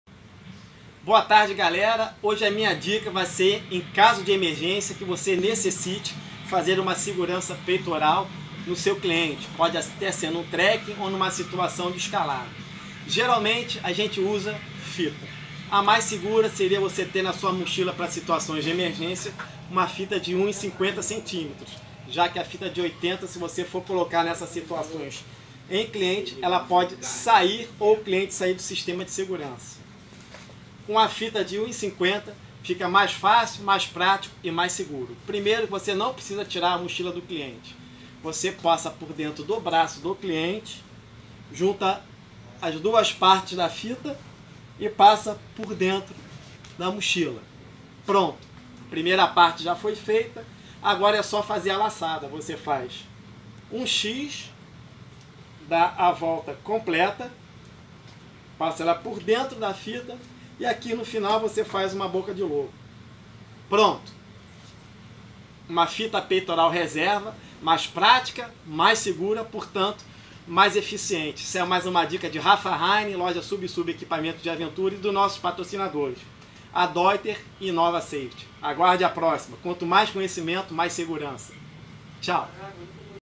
Dicas de segurança na hora da emergência
Cinto de segurança peitoral com uma fita de1.50cm serve pra emergências em rapel, escalada e trekking.